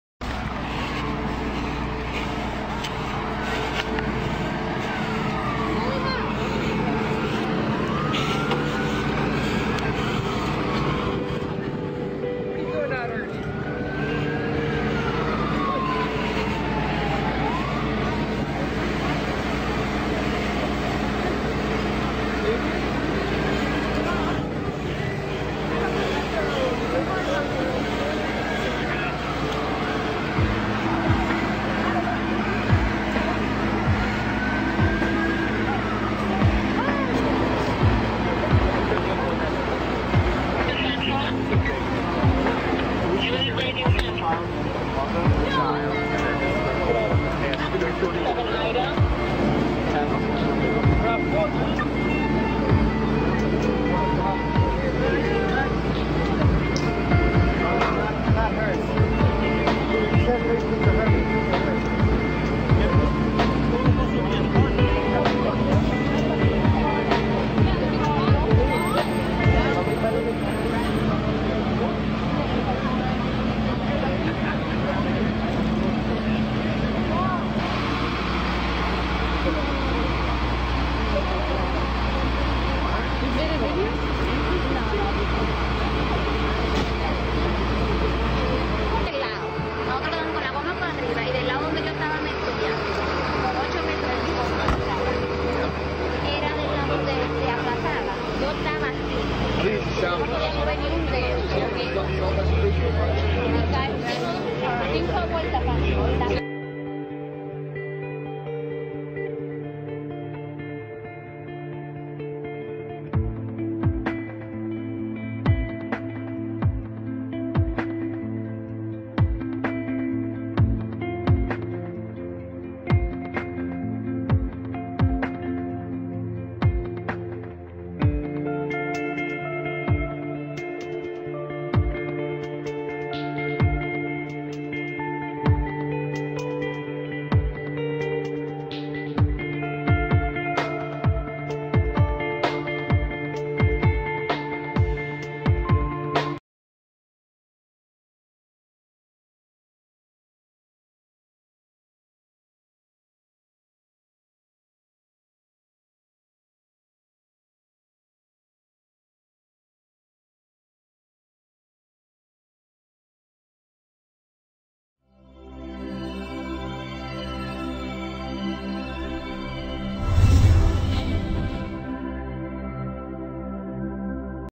Crazy Car Accident in Queens NY SUV crash and flip over woman and baby Survive! 9 26 2014
Headlines Today - Indias Leading News Channel that gets you the Latest News Headlines, Breaking News from India, World, Sports, Entertainment and Bollywood.